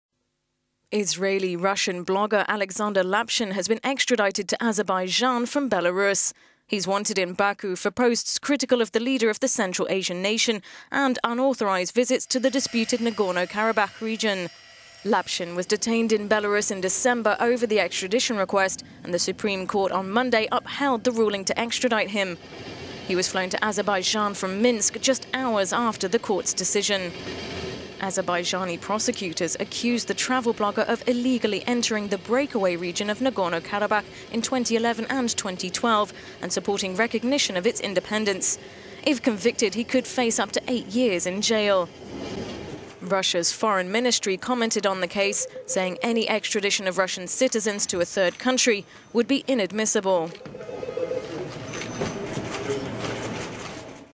Russian-Israeli blogger extradited to Azerbaijan to face charges of insulting the leader
Israeli-Russian blogger Alexander Lapshin has been extradited to Azerbaijan from Belarus, despite objections from Russia and Israel.

He is wanted in Baku for posts critical of the leader of the Caucasus nation and unauthorised visits to the disputed Nagorno-Karabakh region.

Lapshin was detained in Belarus in December over the extradition request. On Monday the Supreme Court upheld the ruling to extradite him.

He was flown to Azerbaijan from Minsk just hours after the court's decision.